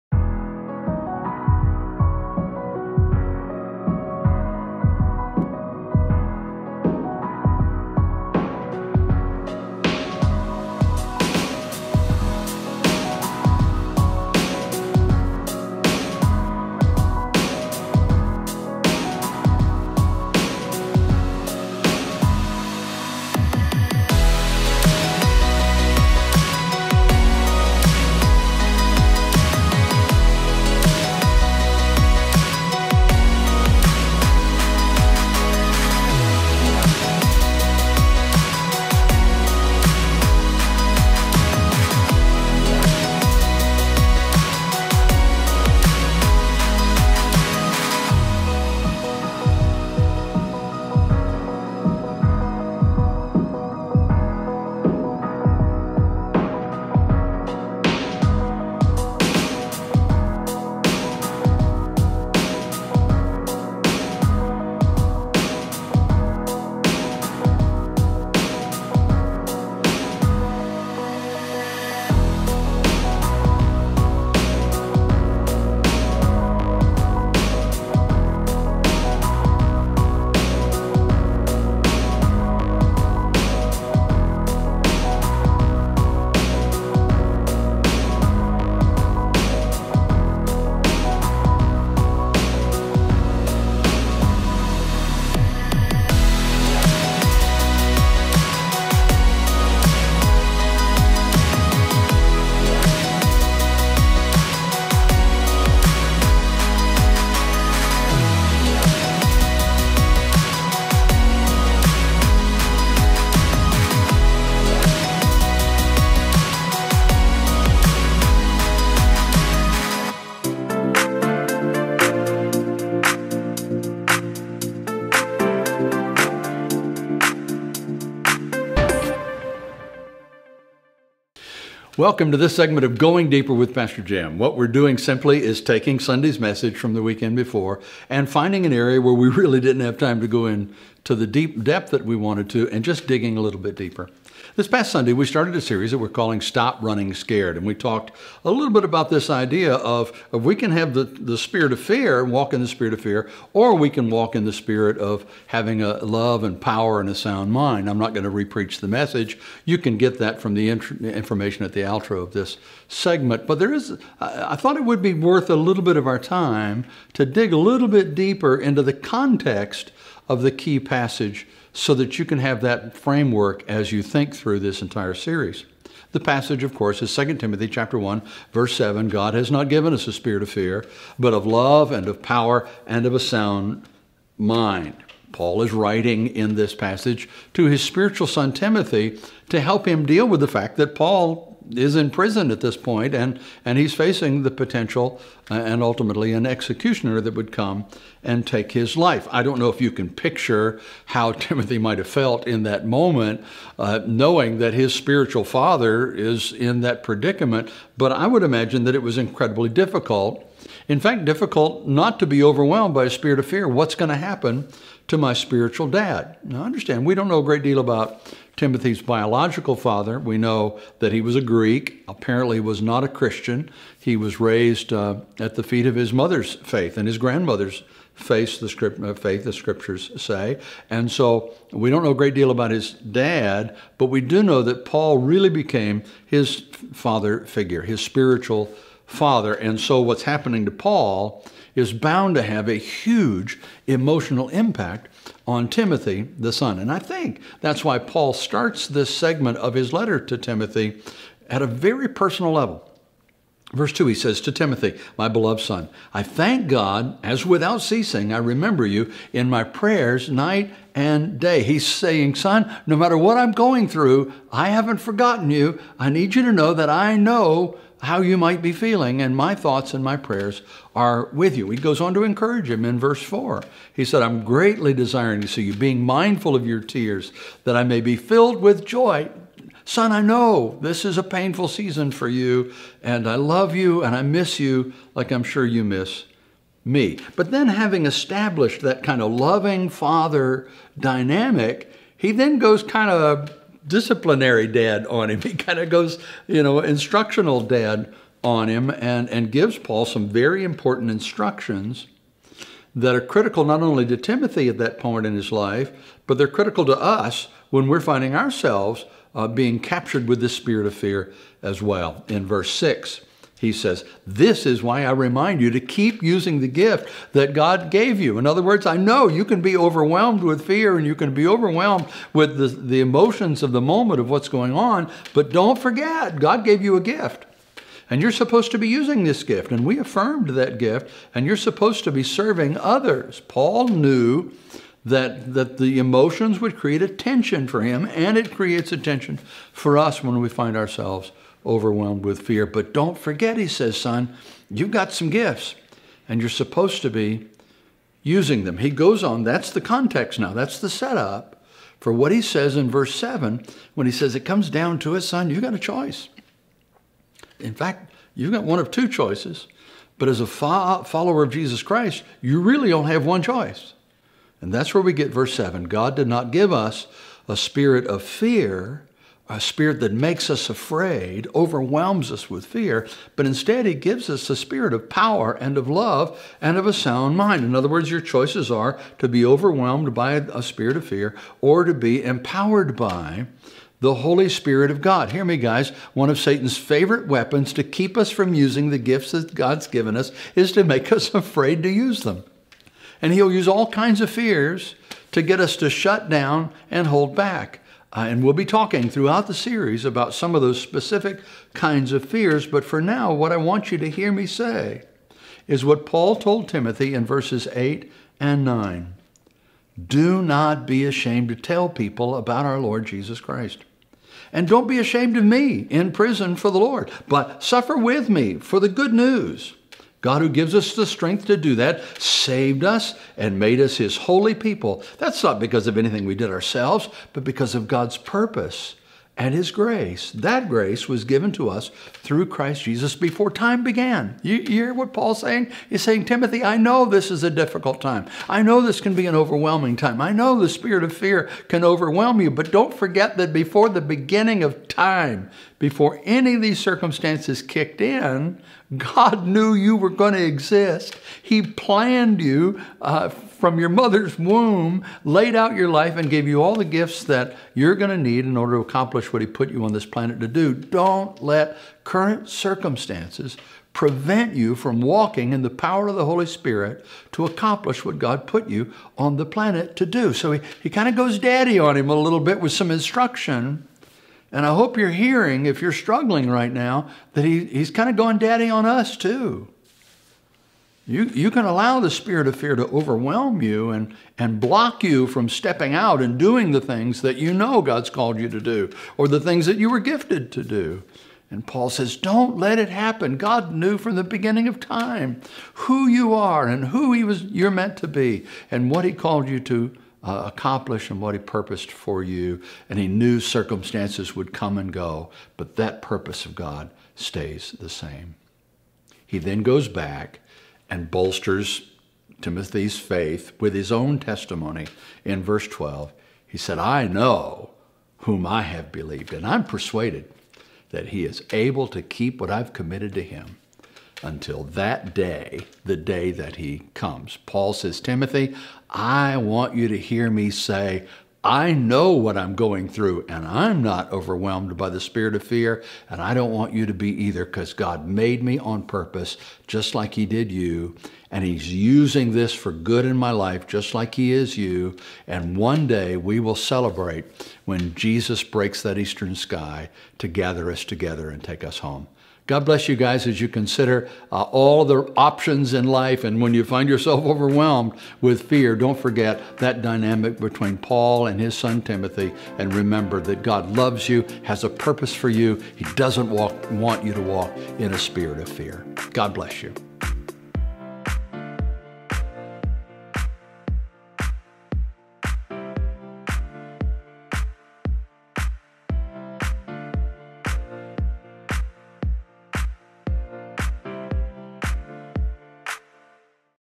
Going Deeper in the Word | Stop Running Scared | Pt. 1
Welcome to Going Deeper, where we'll dive deeper into Sunday's message. This week we'll be discussing into the issue of fear. View ‘Going Deeper in the Word’ Tuesday nights at 7:00pm or on-demand at Facebook
If you would like to see the message from Sunday,